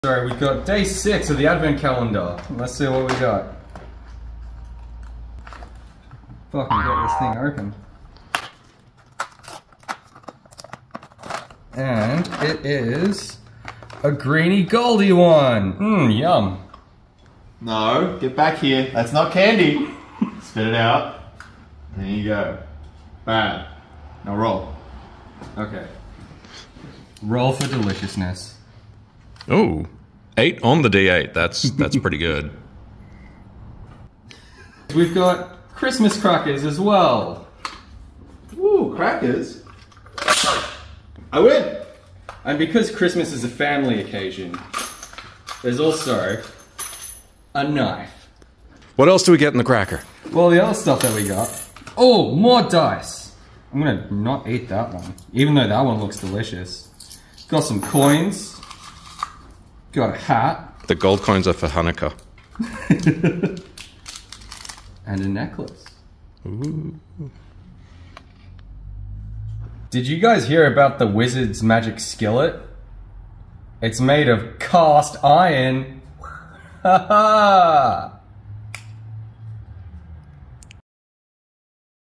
Dicemas Day 6
With Jake out for their RDO it's up to Adonis to see if the rpg adventcalendar is dice or candy today!
We also legitimately have these Christmas crackers for sale from the OSAS website, and we don't just mean Daniel and aDonis (or as we don't call them, DnD) !